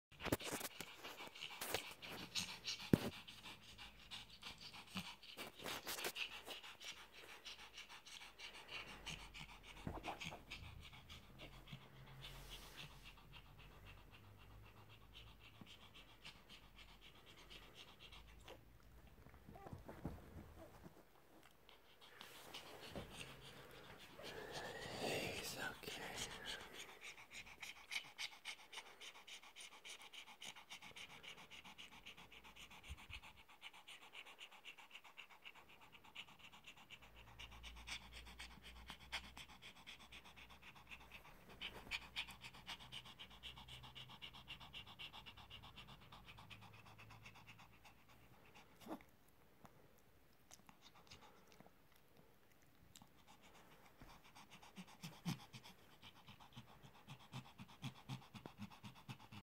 Pomeranian with suspected syringomyelia
This is Fluffy, a 4-year-old Pomeranian with suspected syringomyelia. He has had an MRI scan and spinal tap performed at the Royal Veterinary College. He is currently being treated with a combination of steroids and painkillers. Thankfully, the treatment is having some positive effects. You can read more about Fluffy and his battle with syringomyelia